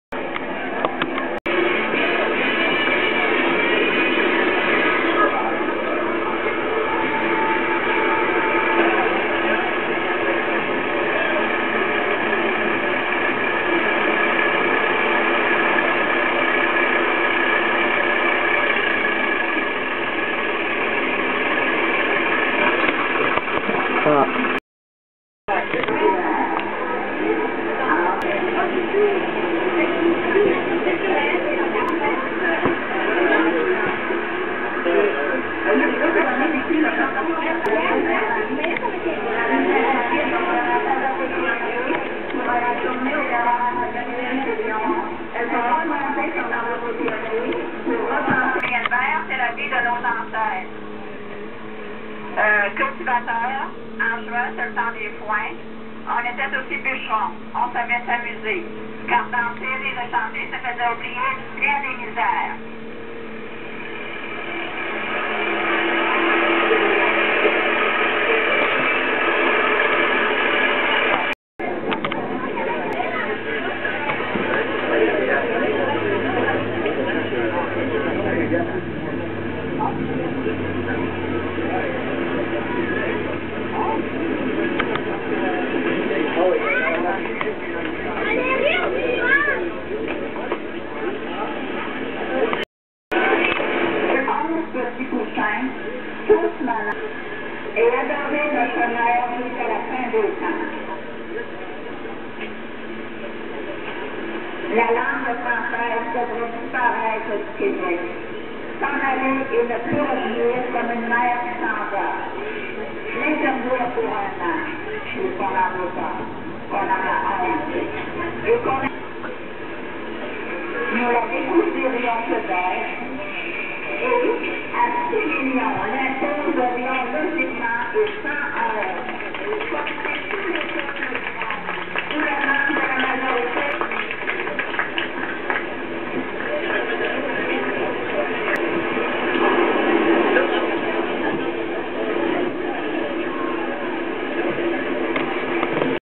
Parade à Sainte-Blandine Rimouski 1989. Fête au village. Chars allégoriques.
Fête au village de Sainte-Blandine en 1989. Désolé, le son et l'image ne sont pas bons. Les extraits proviennent d'une bande VHS. Mon frère Martin à filmé ces images et je pense que ce sont des souvenirs intéressant à partager.  C’est la fête au village.  Cependant, si pour une raison ou une autre, quelqu’un désire que ce vidéo soit retiré, avisez moi et je procéderai.